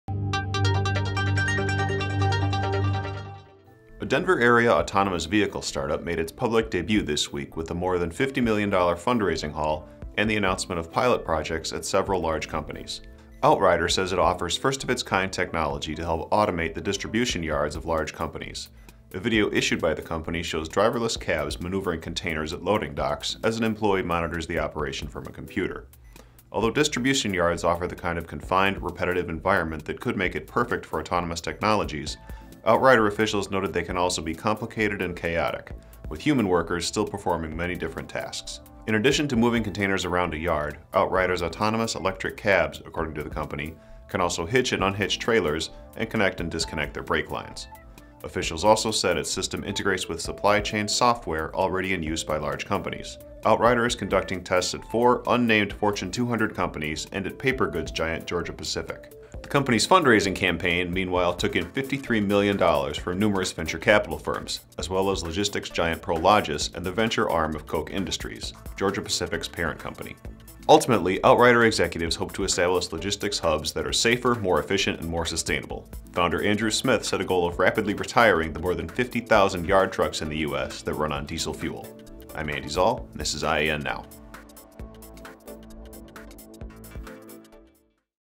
IEN NOW: Startup Launches Autonomous Logistics Trucks
A Denver-area autonomous vehicle startup made its public debut this week with a more than $50 million fundraising haul and the announcement of pilot projects at several large companies.

Outrider says it offers first-of-its-kind technology to help automate the distribution yards of large companies. A video issued by the company shows driverless cabs maneuvering containers at loading docks as an employee monitors the operation from a computer.

Although distribution yards offer the kind of confined, repetitive environment that could make it perfect for autonomous technologies, Outrider officials noted they can also be complicated and chaotic, with human workers still performing many different tasks.

In addition to moving containers around a yard, Outrider’s autonomous, electric cabs, according to the company, can also hitch and unhitch trailers and connect and disconnect their brake lines.

Officials also said its system integrates with supply chain software already in use by large companies. Outrider is conducting tests at four unnamed Fortune 200 companies and at paper goods giant Georgia-Pacific.

The company’s fundraising campaign, meanwhile, took in $53 million from numerous venture capital firms as well as logistics giant Prologis and the venture arm of Koch Industries — Georgia-Pacific’s parent company.

Ultimately, Outrider executives hope to establish logistics hubs that are safer, more efficient and more sustainable — founder Andrew Smith set a goal of rapidly retiring the more than 50,000 yard trucks in the U.S. that run on diesel fuel.